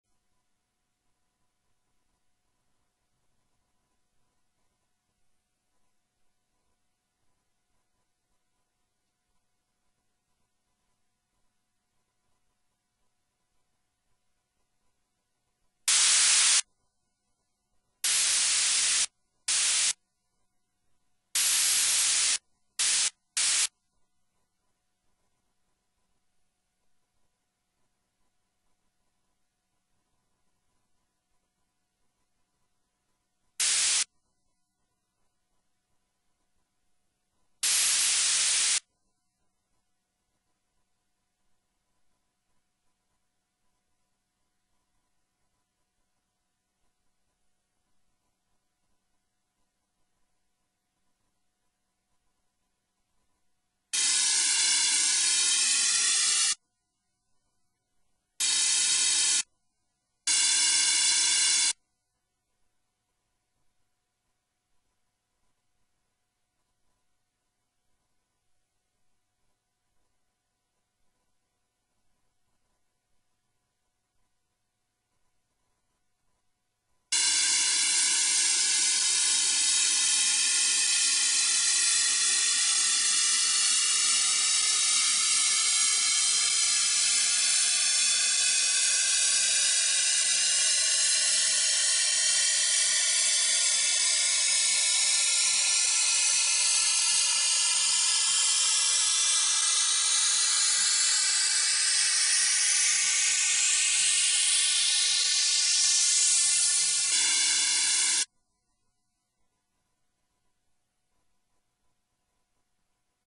FL Studio Tutorial 1 : How to make a never ending uplifter/sweep like SHM and Nicky Romero
READ DESCRIPTION !

Here's a nice uplifter that I made in FL 9. I used Blue Cat's Flanger vst as effect.
Enable to let the vst work you need to download the right version. For my (Windows 7 x64) that was Win VST.